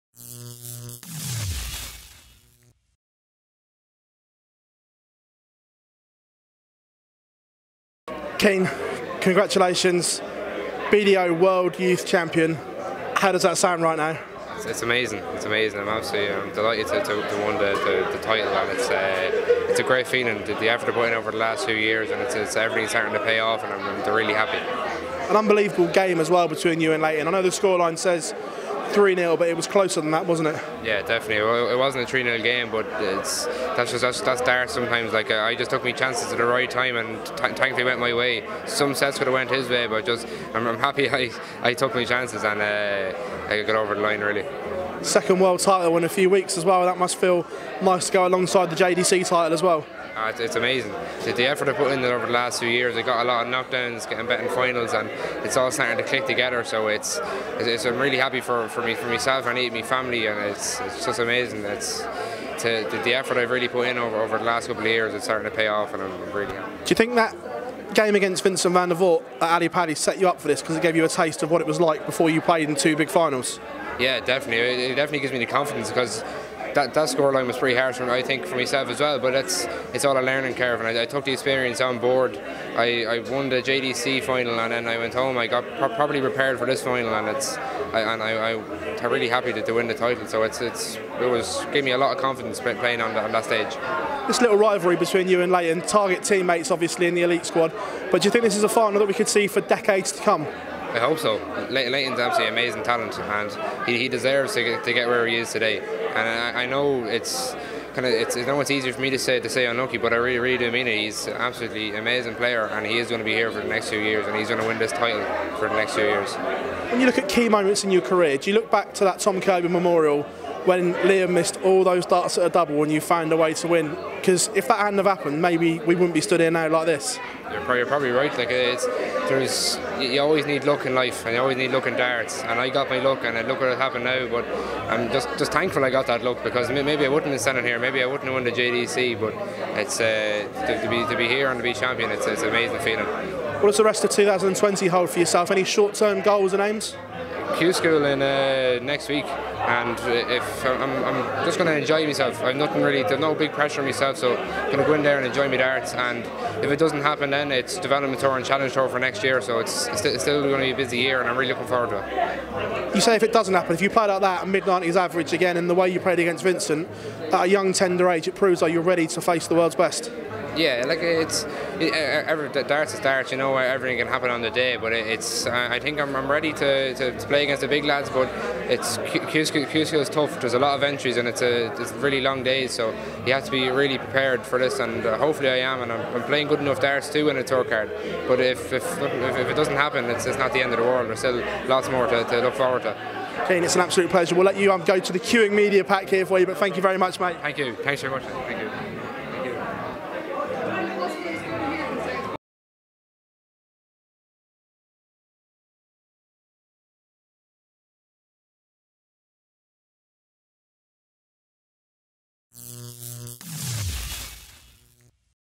Keane Barry: "I think I'm ready to play against the big lads, but Q School is tough"
Irish ace Keane Barry reacts to his 3-0 victory over Leighton Bennett to be crowned 2020 BDO World Youth Champion and also outlines his plans for the year ahead, including a trip to Qualifying School.